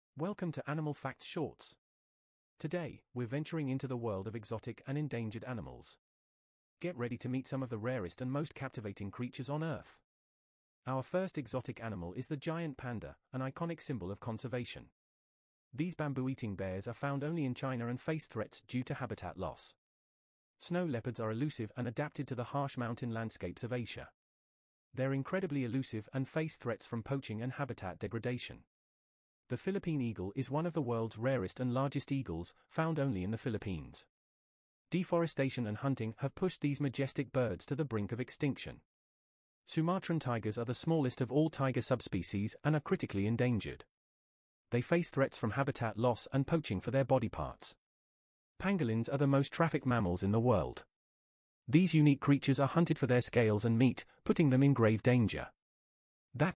🌍 "Guardians of the Wild: Exotic & Endangered Animals" 🦋
🦏 Journey into the remarkable world of exotic and endangered animals with our latest Animal Facts Shorts video, "Guardians of the Wild: Exotic & Endangered Animals"! From the iconic giant panda to the elusive snow leopard, we're shedding light on these rare and incredible creatures facing the threat of extinction. 🌏🐾

Did you know the Philippine eagle is one of the rarest eagles on Earth, or that Sumatran tigers are critically endangered? Join us in celebrating their beauty and raising awareness for their conservation.

Welcome to "Animals in a Flash"! In this 30-second short, we're diving into the world of adorable pandas. Watch as these playful giants frolic, munch on bamboo, and showcase their charming antics. Pandas are not only incredibly cute but also vital to wildlife conservation efforts. Learn more about these incredible creatures and their role in the animal kingdom.